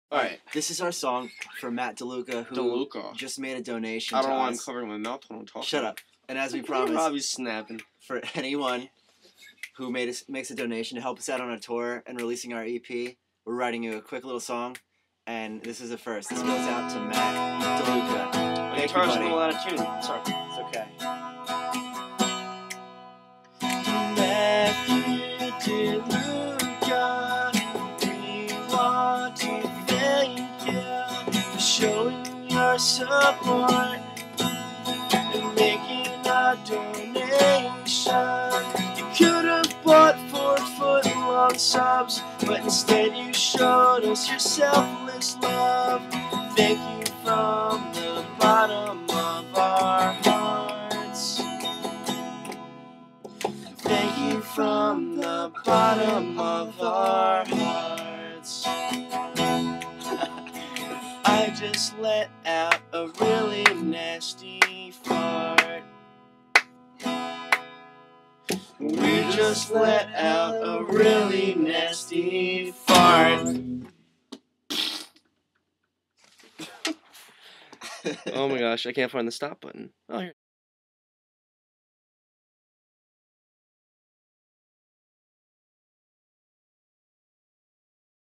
shd 1
video we made for Matt Deluca who made a donation to us through anyone who makes a 5 dollar donation, we will write and record a song for them!!!